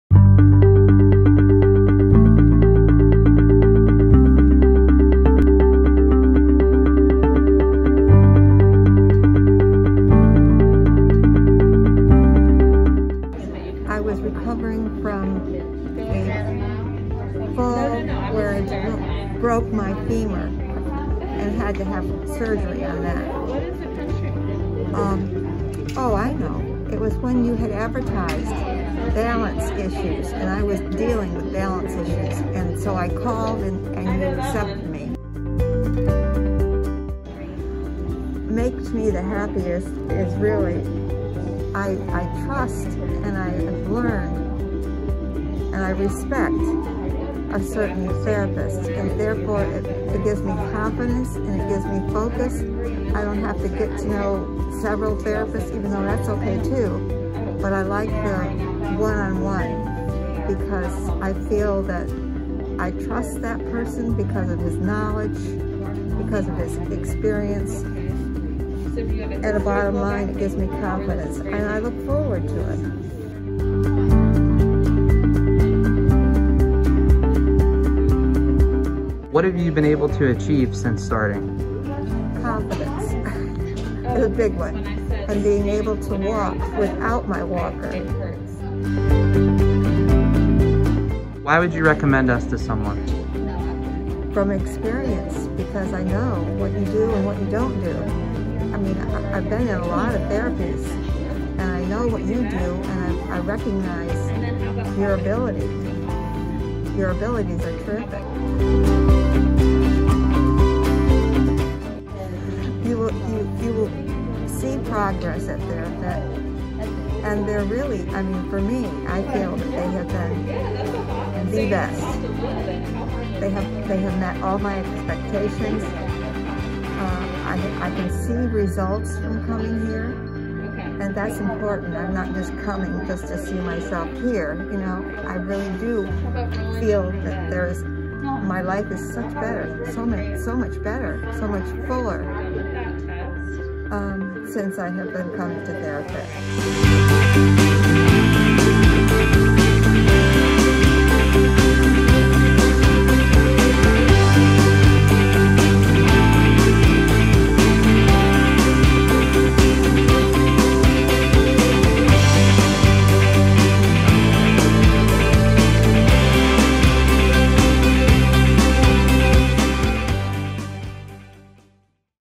Elaine Gets Her Balance Back
After surgery on her leg and struggling with balance, Elaine came to Therafit Rehab.